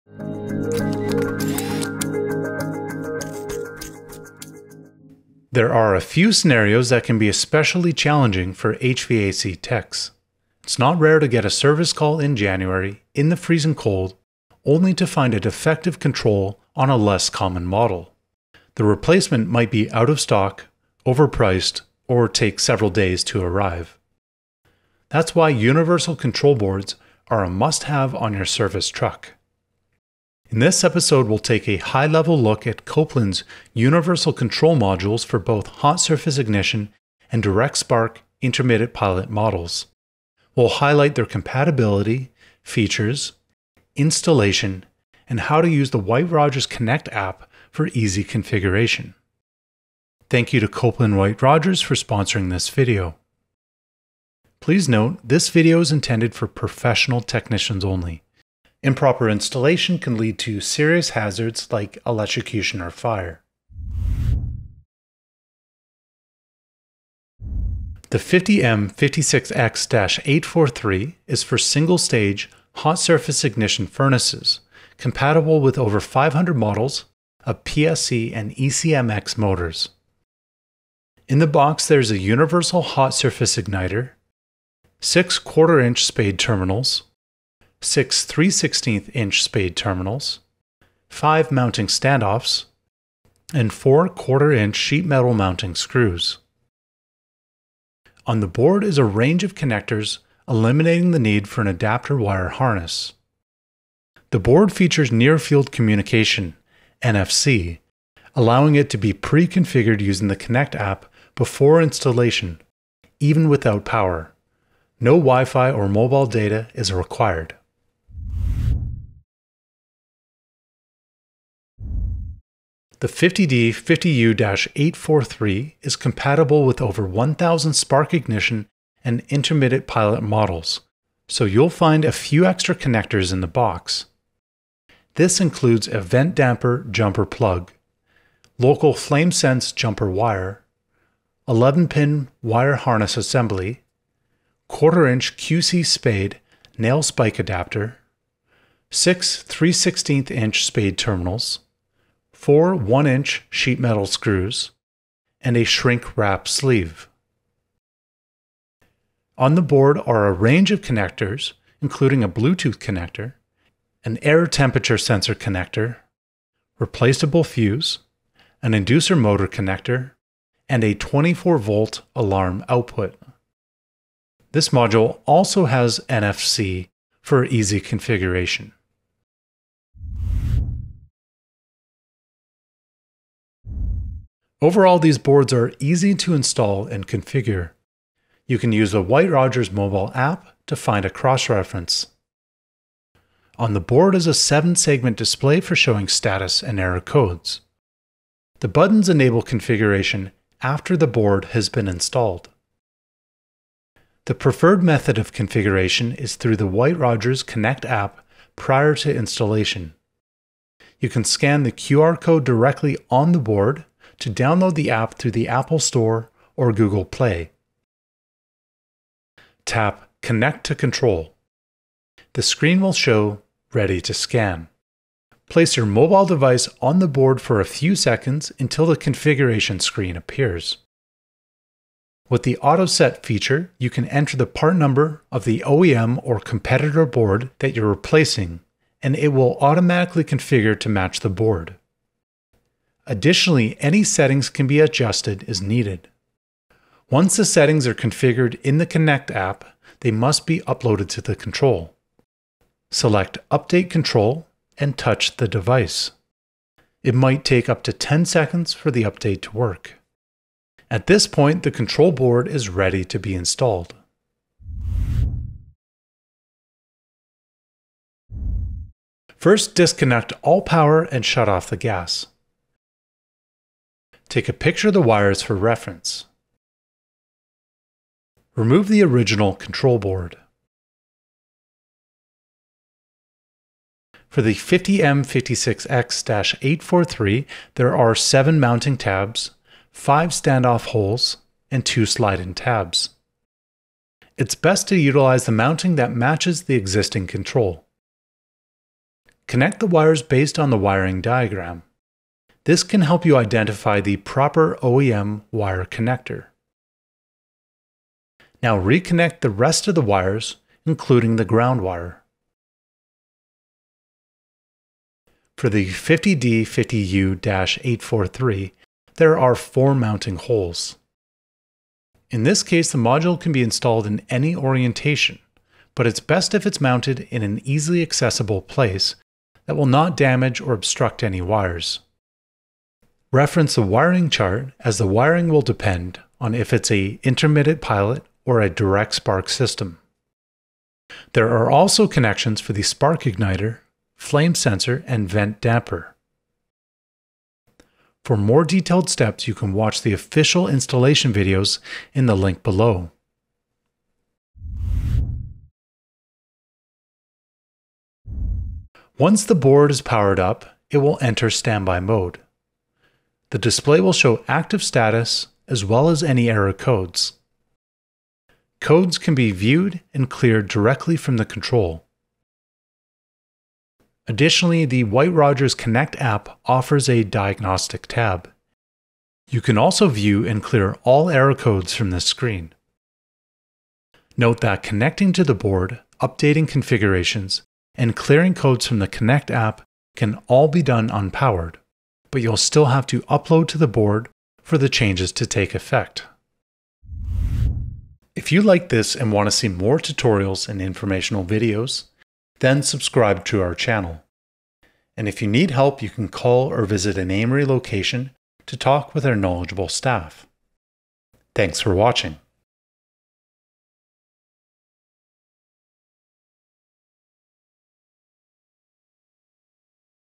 Universal Control Boards - White Rodgers by Copeland | Repair & Replace
In this episode of Repair & Replace, we take a high-level look at two powerful universal control boards from White Rodgers by Copeland, must-haves for any HVAC service truck.

The 50M56X-843 is an Integrated Furnace Control designed for single-stage hot surface ignition (HSI) systems with PSC or ECMx motors.
The 50D50U-843 "All-Spark" is compatible with over 1,000 direct spark and intermittent pilot ignition systems.

Together, these modules replace over 1,500 existing OEM boards, providing a flexible, reliable, and efficient solution in the field, especially when time and availability are critical.

Features include:
- 7-Segment display for status and error codes
- Included wire harness and a range of connections for easy installation
- Various mounting options
- Complete Hardware Kits Included
- NFC Preconfiguration using the Connect App
- User-Friendly Diagnostics

Both control modules come equipped with Near Field Communication (NFC) and can be configured with a smartphone using the White Rodgers Connect App.

Thank you to White Rodgers by Copeland for sponsoring this video.

Note: This video is intended for professional HVAC technicians only.

0:00 Intro
1:04 50M56X-843 Single Stage HSI
1:59 50D50U-843 All-Spark
3:00 Connect & Configure
4:25 Installation
5:55 Diagnostics